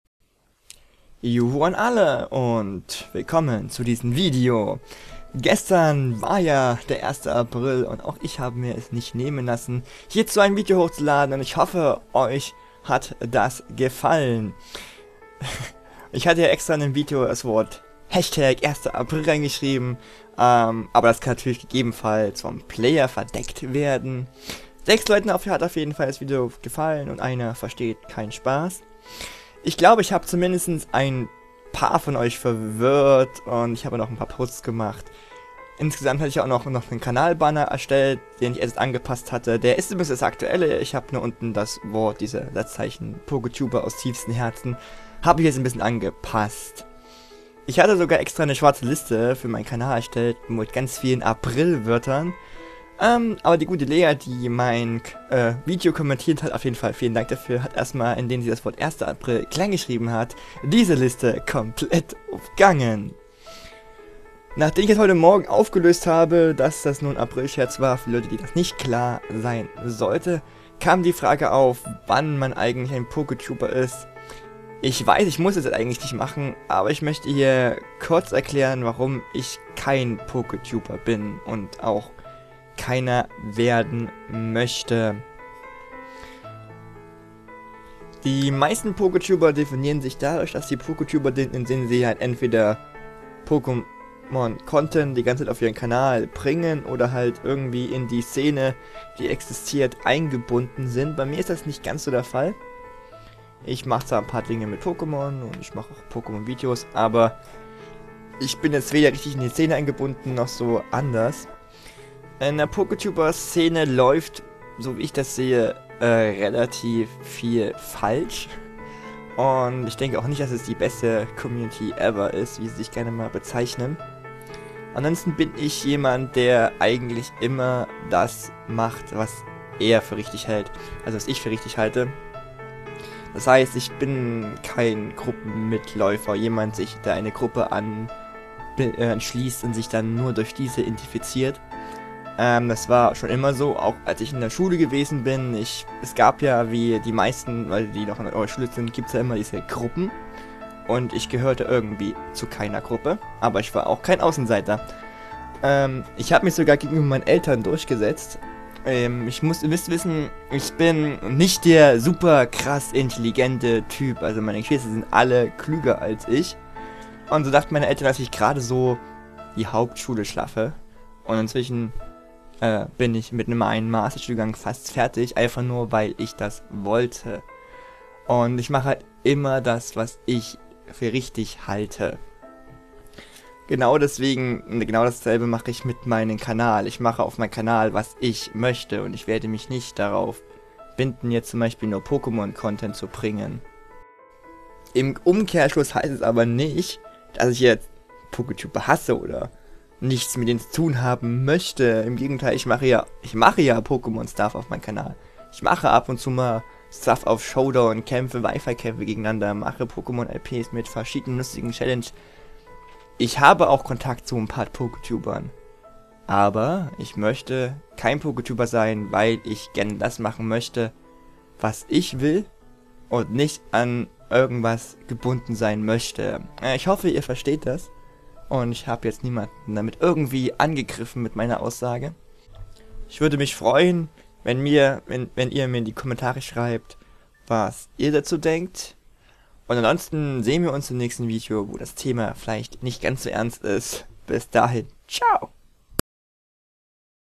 Aprilscherze Resume - Warum ich kein PokeTuber bin
Im Video wird alles gesagt. Ich hoffe ihr versteht und akzeptiert das.

Das ist das erstmal das ich was aus meine "private Leben" erzähle und ich hoffe das wird das letzte mal sein. Aber ich denke es war notwendig damit die meisten es verstehen.